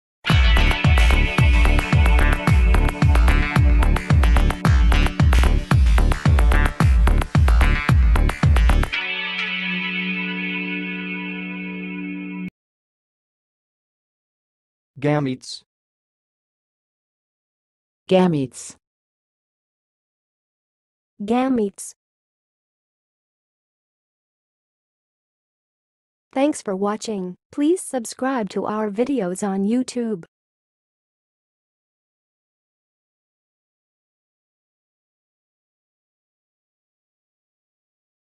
How to say "gametes"! (High Quality Voices)
Watch in this video how to say and pronounce "gametes"!